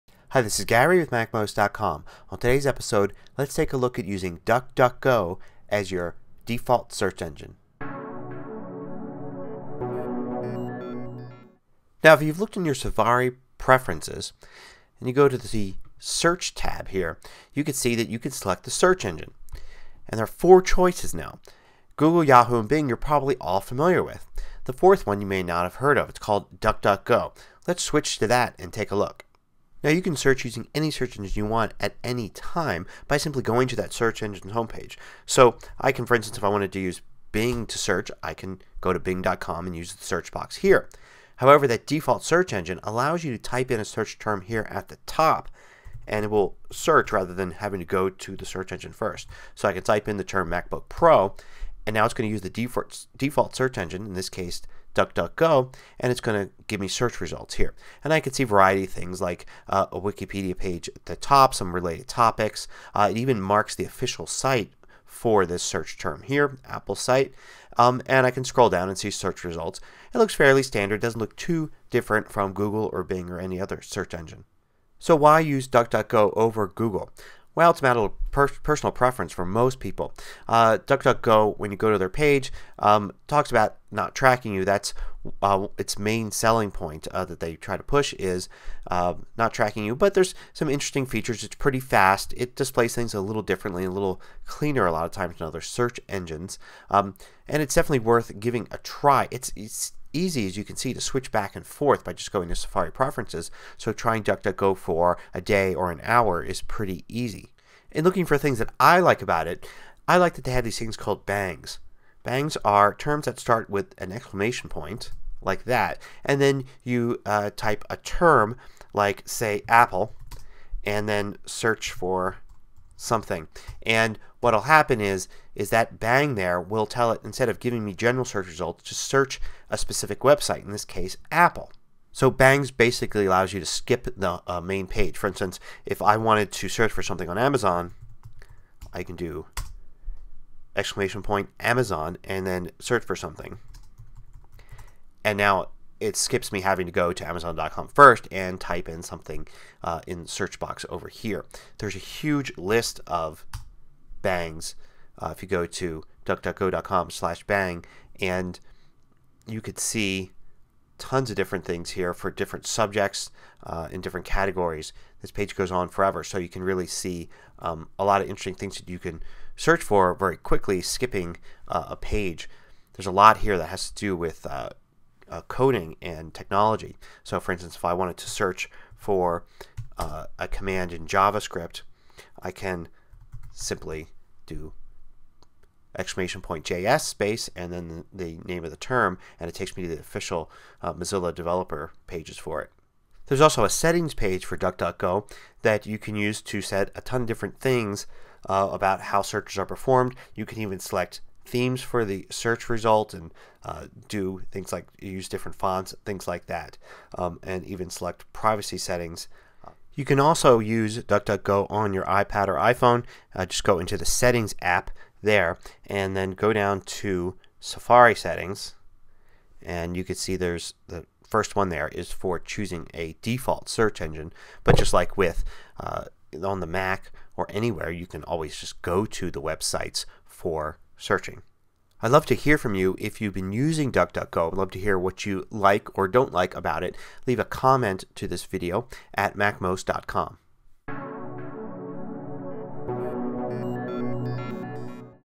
Using DuckDuckGo As Your Default Search Engine (#1074)
You can change your default search engine in Safari to Google, Bing, Yahoo! or DuckDuckGo. The latter offers no-track searching and a variety of other features. You can use "bangs" like !apple to quickly search inside popular websites. You can also set it as your default on iOS.